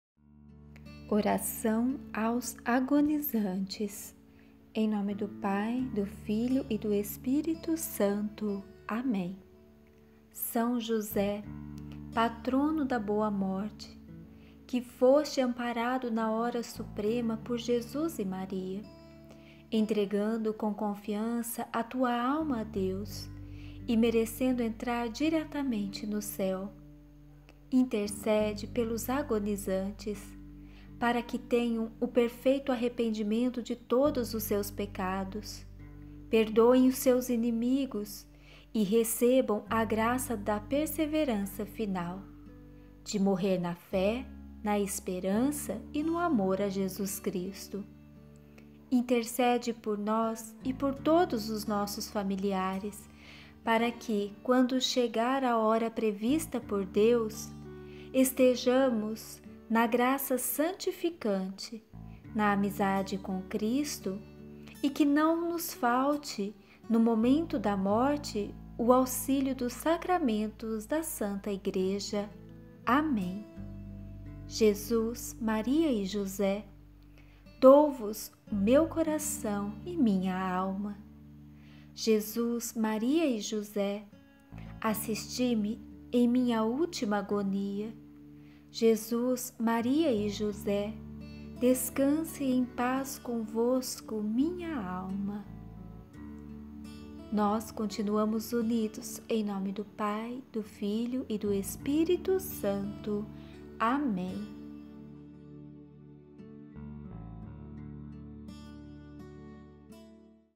Oração Pelos Agonizantes
Vários Santos da nossa Igreja relatam o quanto é importante rezar pelos agonizantes.
É uma hora muito difícil, pois o demônio sabe que o seu tempo está acabando e ele precisa tomar essa alma para si.
A oração traz força, paz para nossos irmãos.
Façamos a nossa parte, pois um dia também precisaremos das mesmas orações!
Deus abençoe!!

ORAÇÃO:
SÃO JOSÉ, PATRONO DA BOA MORTE, que foste amparado na hora suprema por Jesus e Maria, entregando com confiança a tua alma a Deus e merecendo entrar diretamente no Céu; intercede pelos agonizantes, para que tenham o perfeito arrependimento de todos os seus pecados, perdoem os seus inimigos e recebam a graça da perseverança final: de morrer na fé, na esperança e no amor a Jesus Cristo. Intercede por nós e por nossos familiares pra que, quando chegar a hora prevista por Deus, estejamos na graça santificante, na amizade com Cristo, e que não nos falte no momento da morte o auxílio dos Sacramentos da Santa Igreja. Amém.
JESUS, MARIA E JOSÉ, dou-vos meu coração e minha alma!
JESUS, MARIA E JOSÉ, assisti-me em minha última agonia!
JESUS, MARIA E JOSÉ, descanse em paz convosco minha alma!